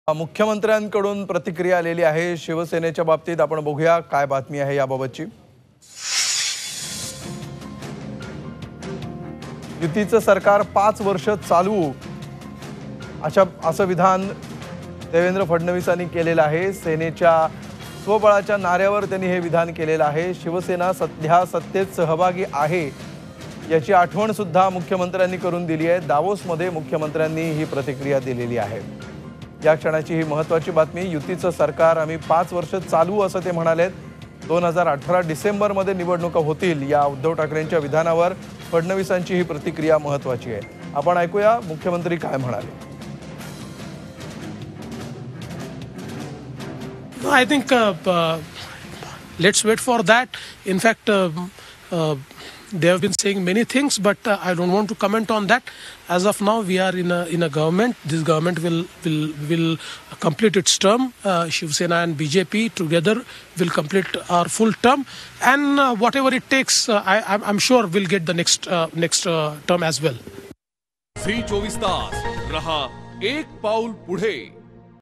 शिवसेनेच्या स्वबळाच्या नार्‍यावर मुख्यमंत्र्यांंची प्रतिक्रिया
Switzerland,Davos CM Devendra Fadanvis Reaction On Shivsena Fight Independent In Election